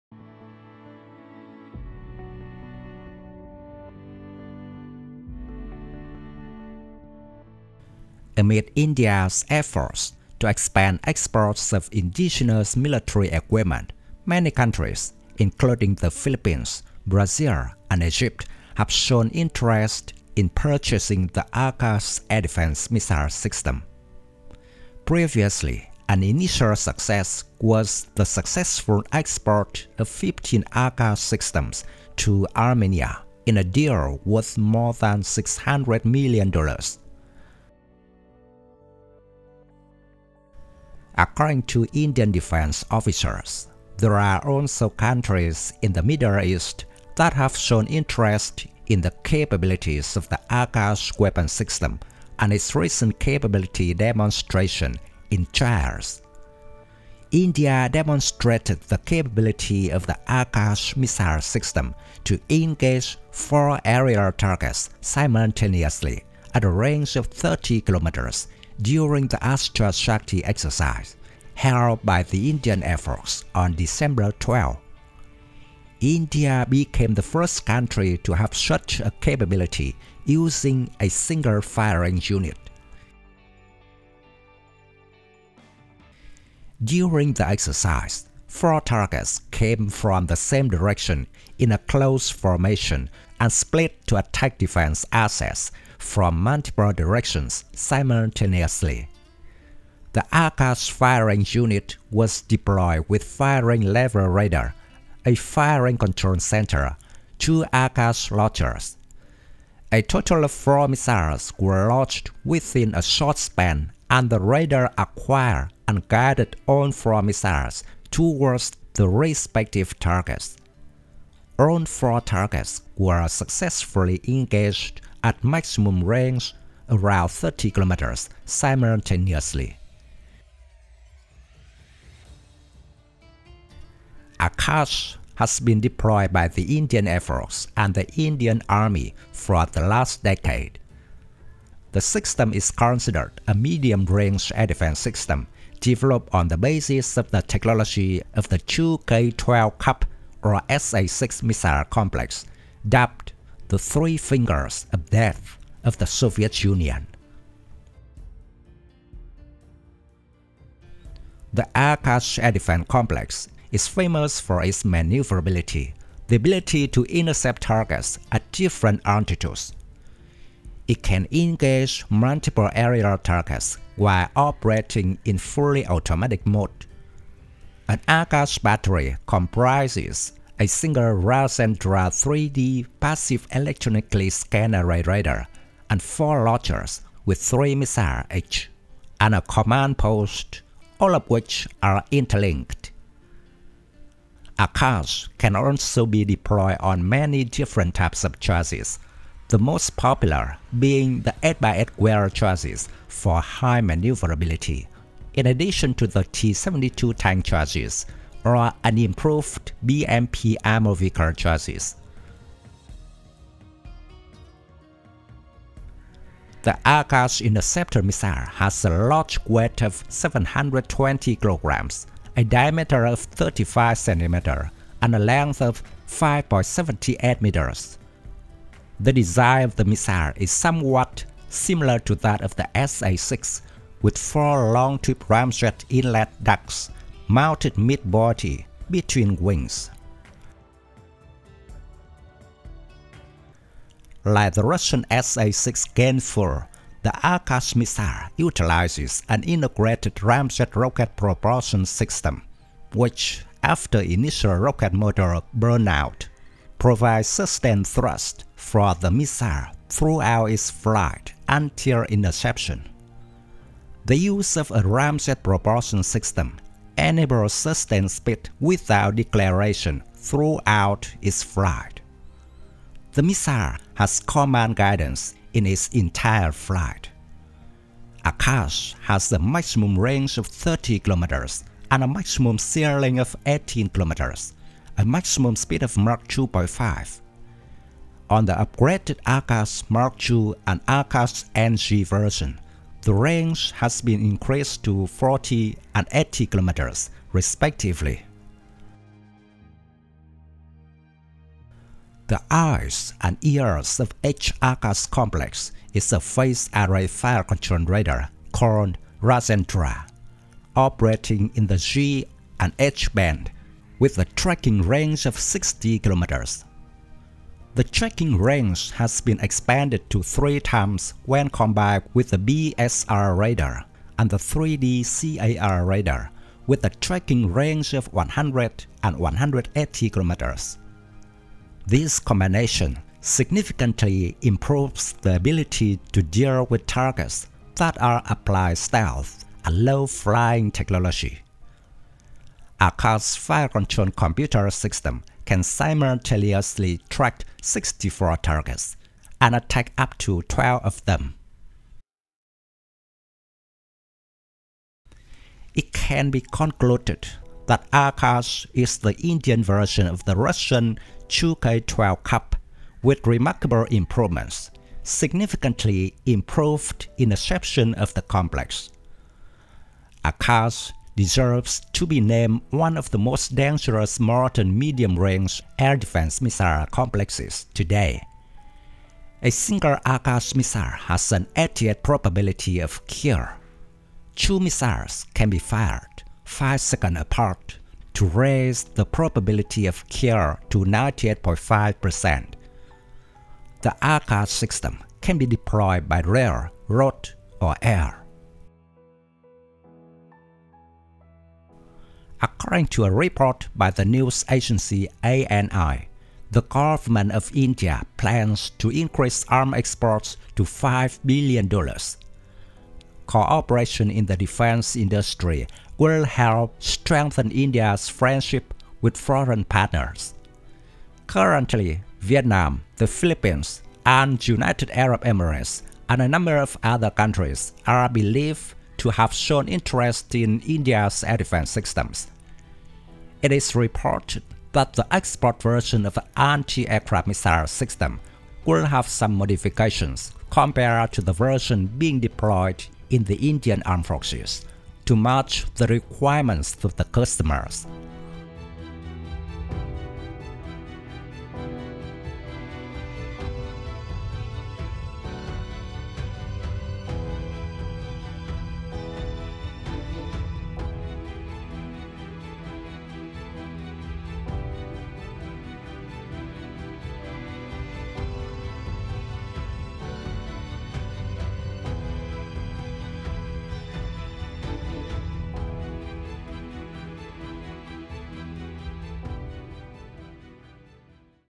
India's Impressive Akash Missile System Received Great Attention
Philippines, Brazil and Egypt, have shown interest in purchasing the Akash air defense missile system. Previously, an initial success was the successful export of 15 Akash systems to Armenia in a deal worth more than 600 million USD. India demonstrated the capability of the Akash missile system to engage four aerial targets simultaneously at a range of 30 kilometres during the Astrashakti exercise held by the Indian Air Force (IAF) on December 12. India became the first country to have such a capability using a single firing unit.

Thank you very much.
Best regards
Dung Tran.